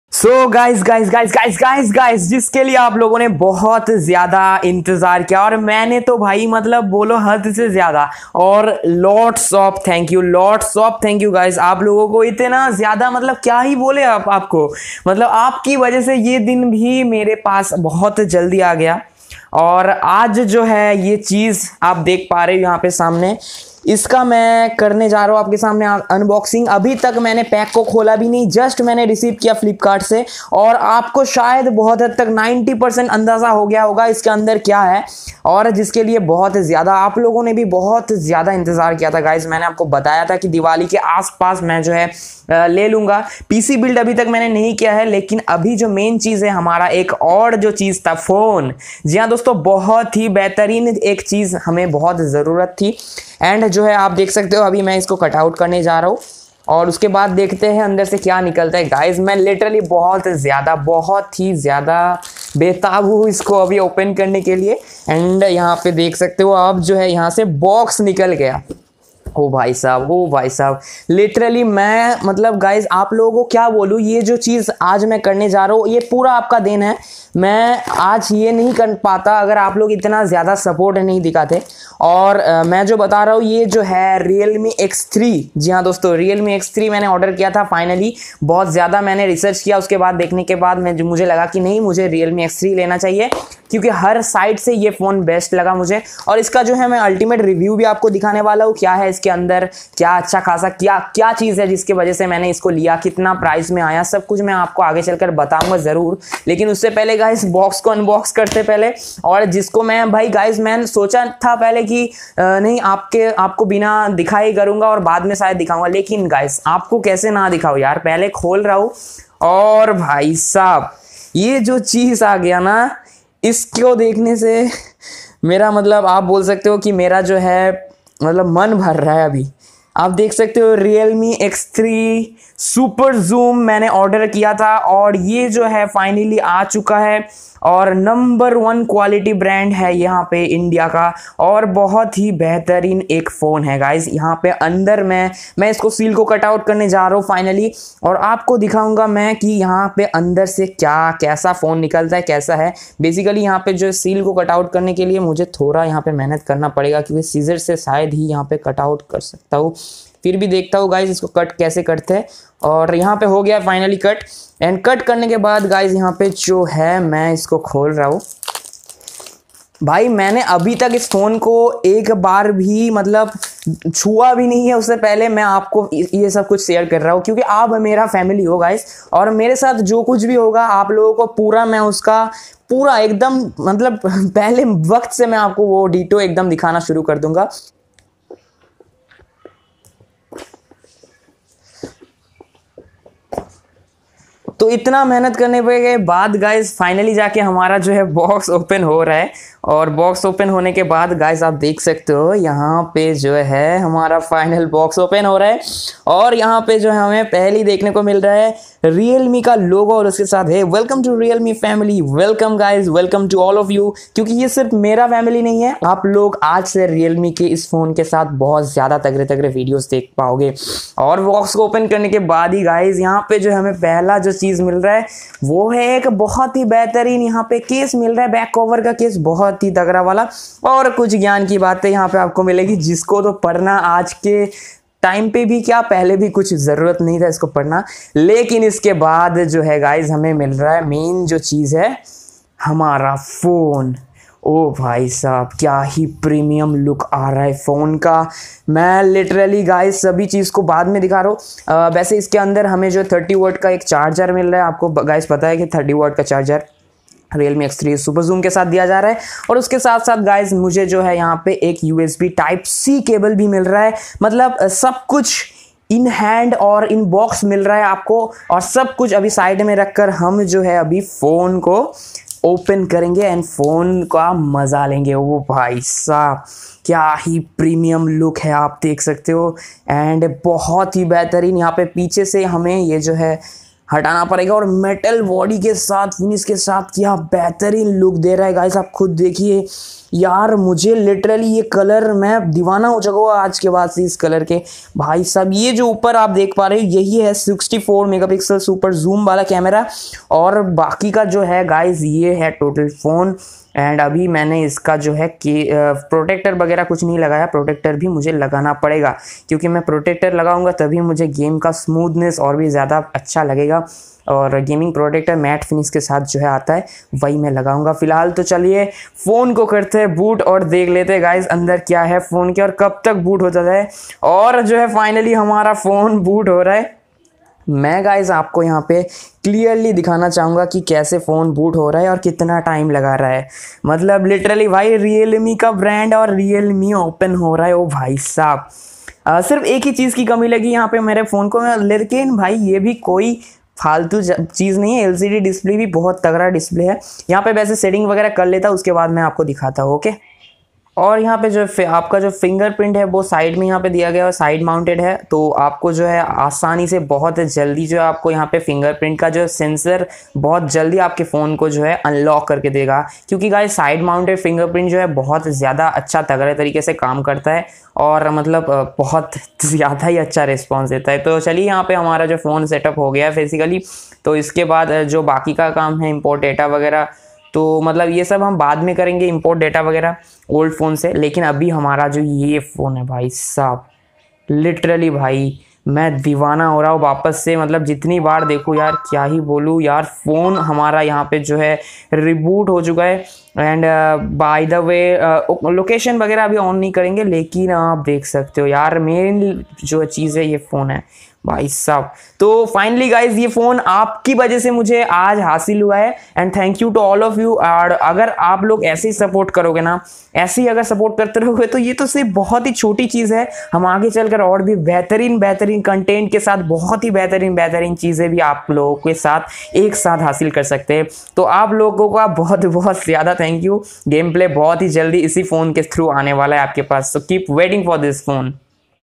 Unboxing My New Phone 🔥 || Realme X3 Superzoom || Thanku Everyone
Unboxing My New Phone 🔥 || Realme X3 Superzoom || Thanku Everyone, Thanks YouTube ||

about this video :

hello dosto,

aaj k iss video mein Maine Unboxing kiya hai mere new phone ka jo ki sirf aur sirf aap logo k liye mai aaj le saka hu aur aap logo ko aur bhi behtar videos de sakta hu.

New Phone : Realme X3 Superzoom

Share, Support, Like!!!

My Mic